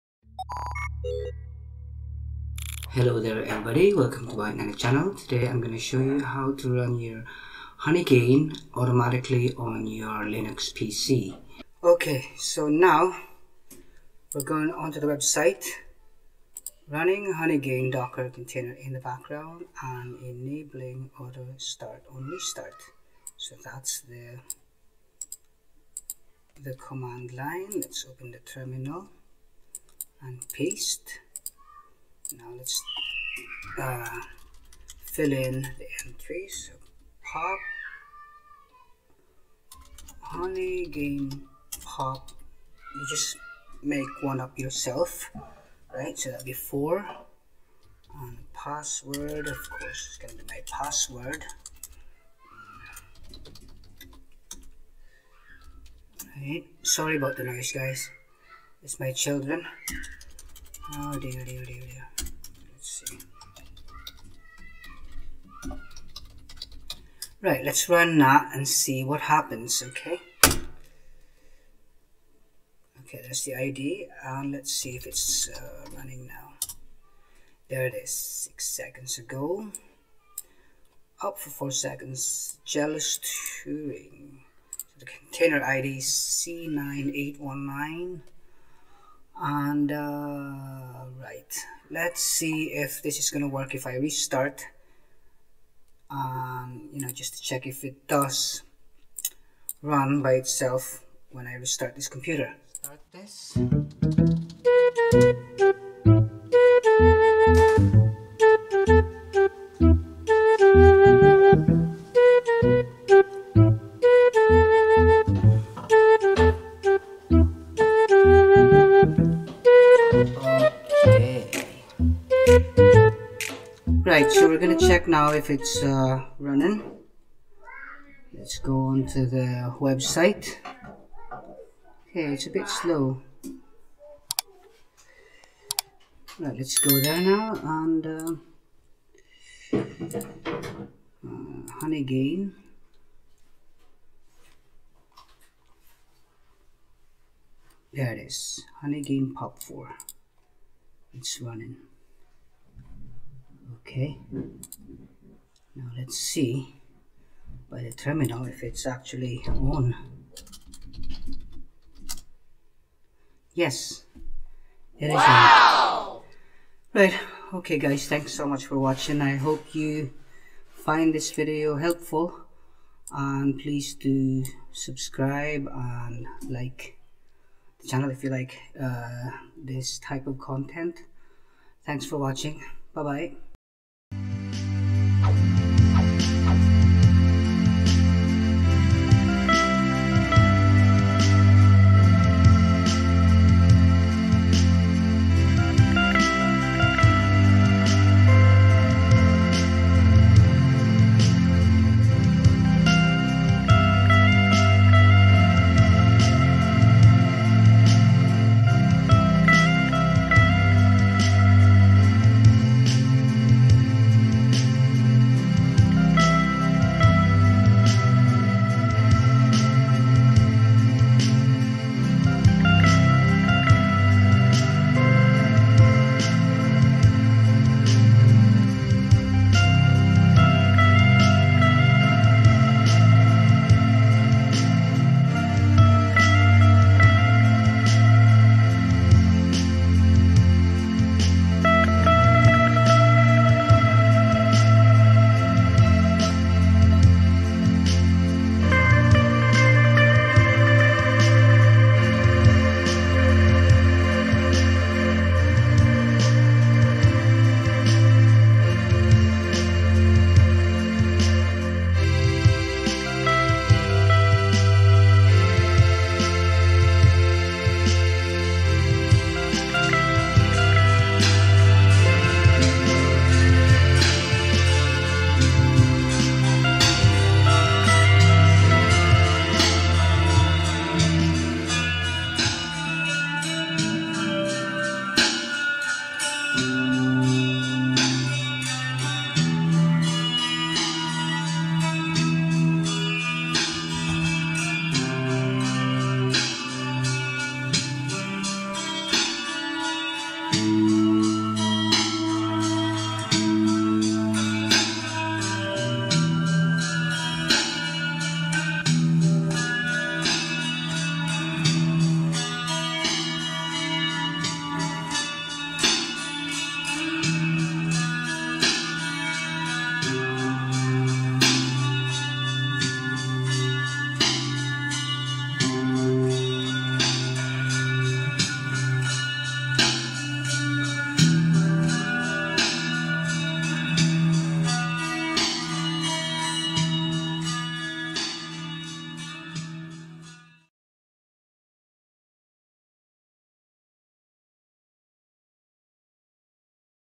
how to auto start honeygain on desktop pc running on linux
Hi there,  here's a very short video on how to auto start honeygain app on a linux run desktop pc without needing to open the terminal emulator and entering the command line each time.

Just copy the code below and enter your email and password accordingly.